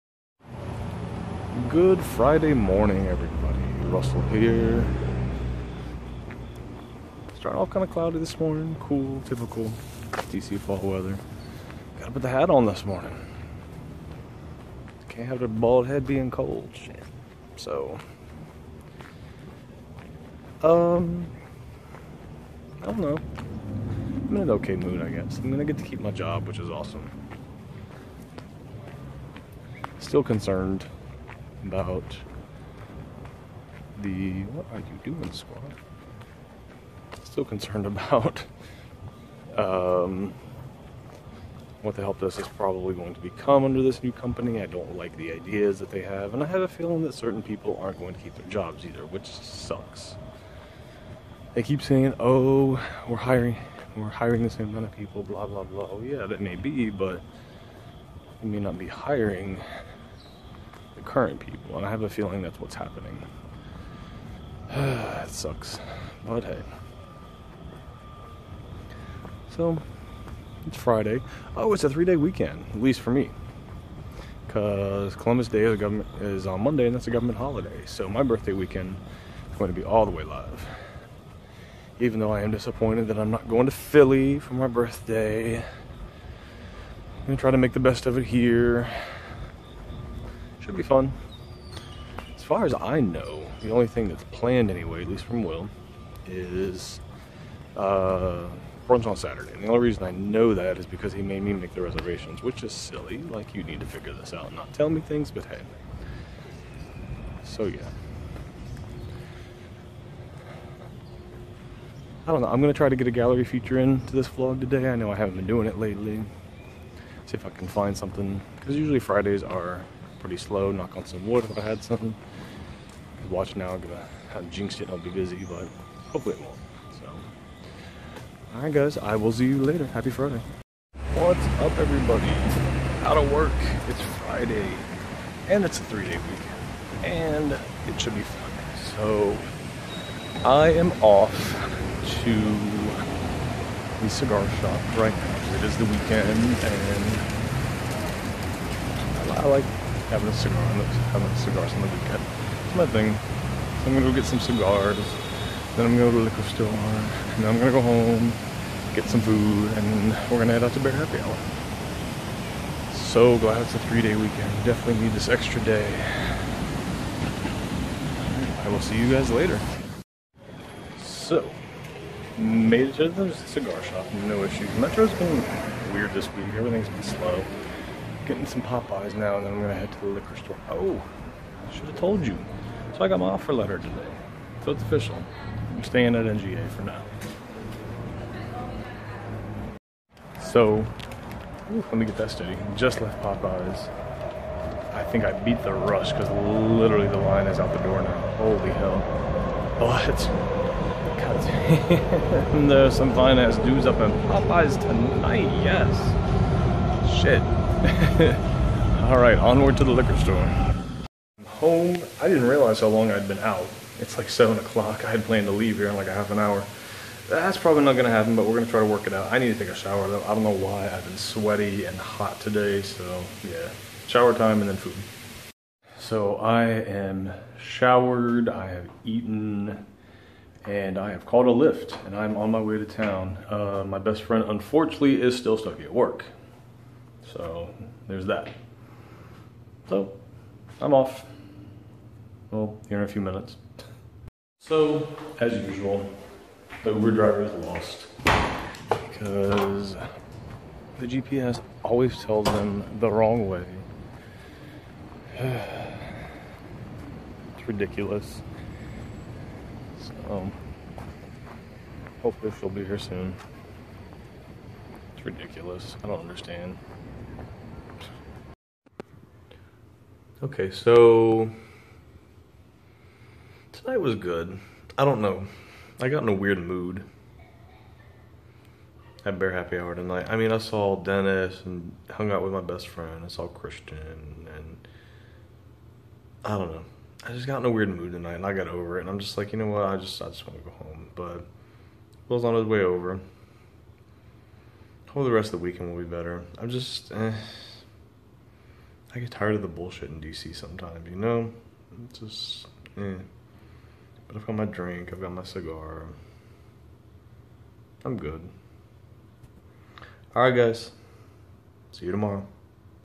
VLOG 29 - I got the offer letter. BHH and overall getting tired of DC
So I officially got the offer letter. Also went to BHH with some friends which put me in a mood.  I'm just tired of the BS.